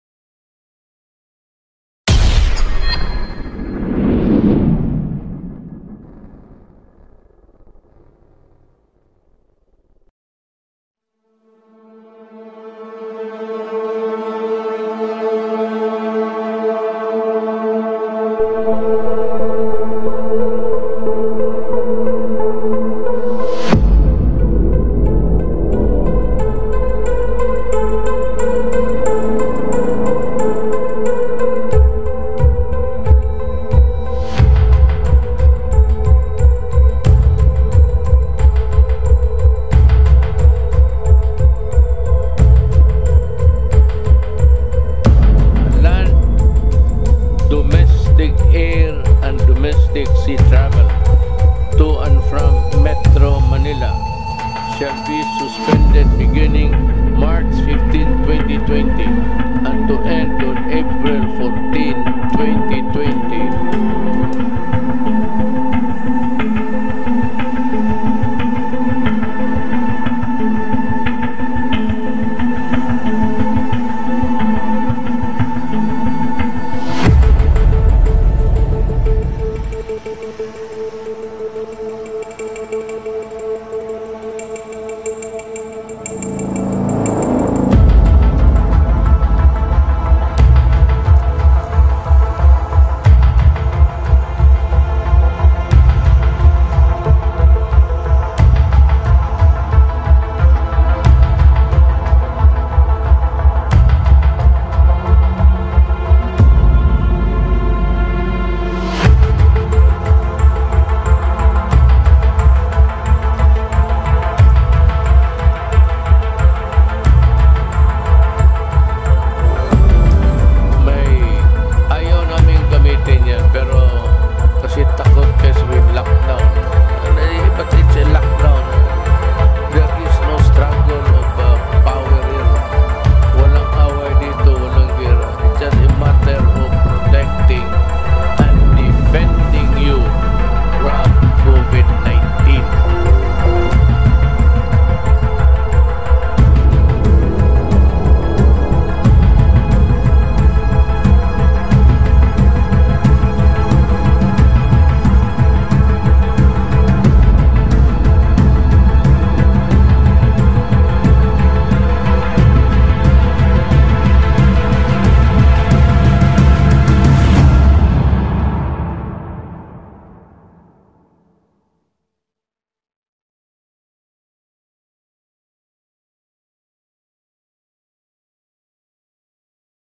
The Lockdown: One Month in Metro Manila (COVID-19 Documentary)
At 10:00 PM on March 15, 2020. Metro Manila get lockdown. This was done to stop a deadly virus from spreading further across the nation. This documentary is dedicated to all those who've been battling tireless again the Covid-19 virus inorder to keep the epidemic at bay.